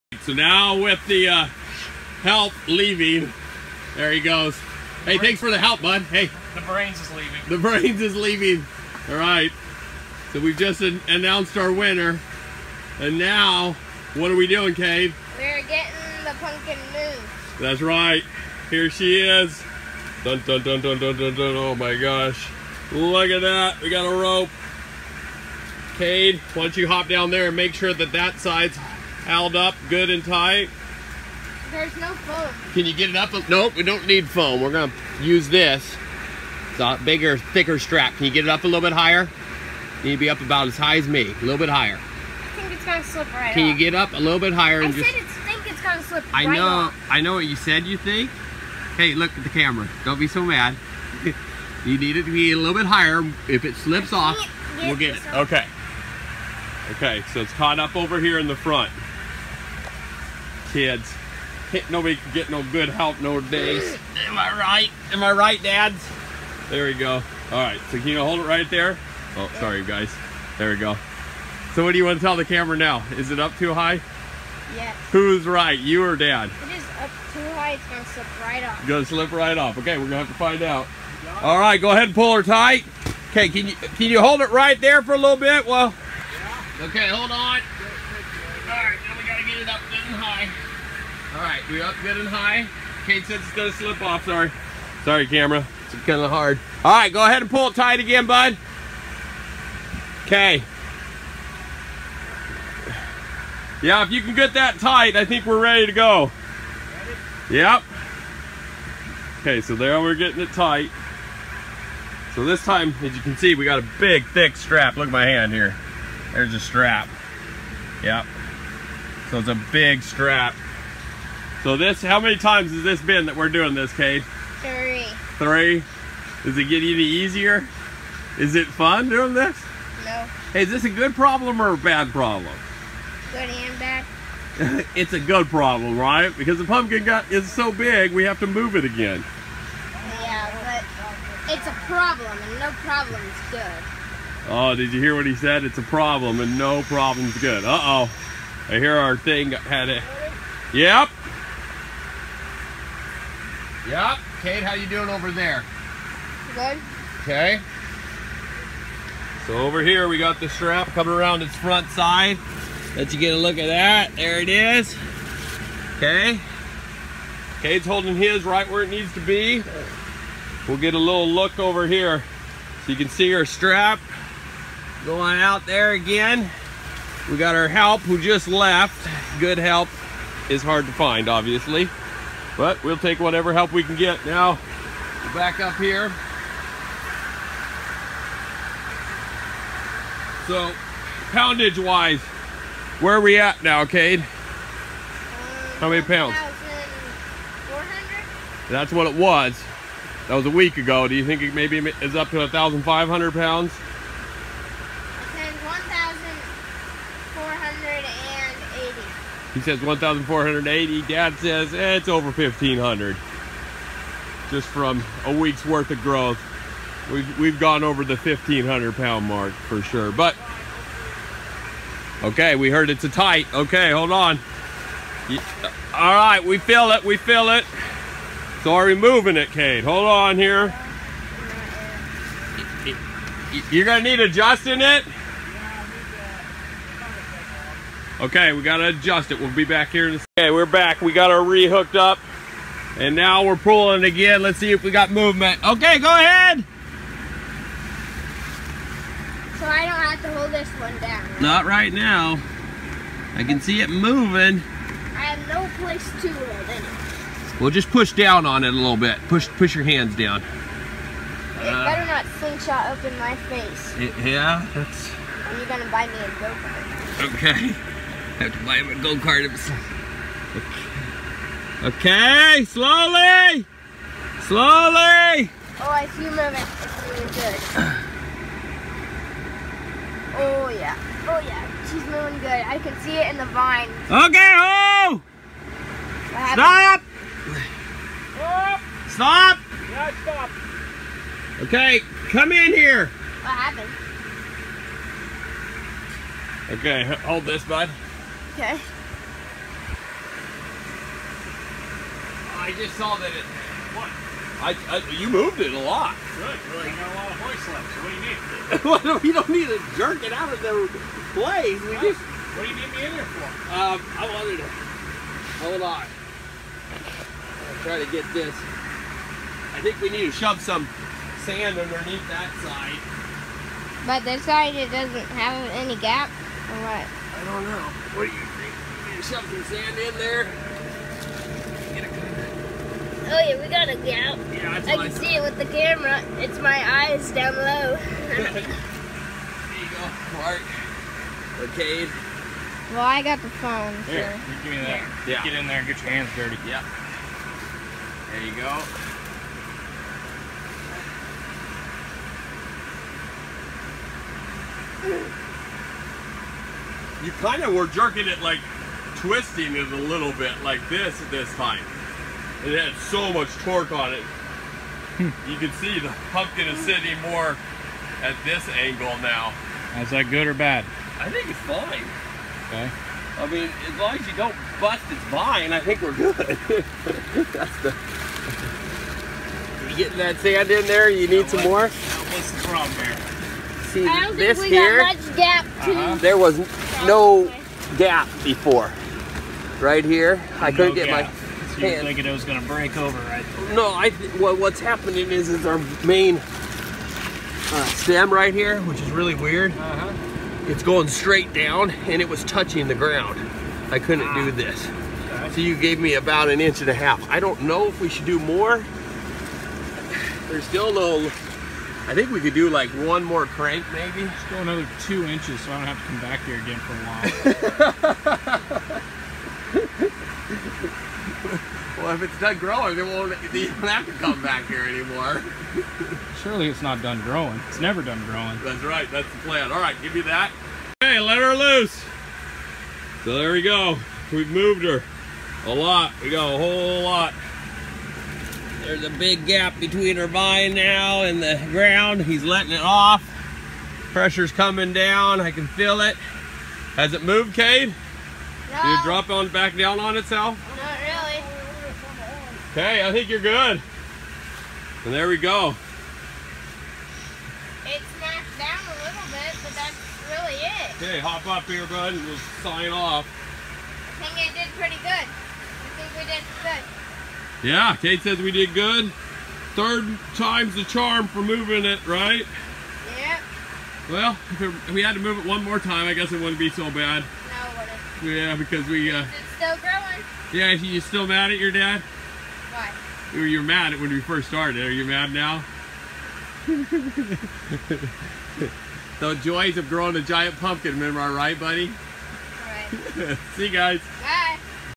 Moving the Giant Pumpkin...again.
Teaching the kid to move a Giant Pumpkin! Does the kid know best or does dad know best...Let's find out.

I haven't had to move a pumpkin before this year and now we are moving it for a third time!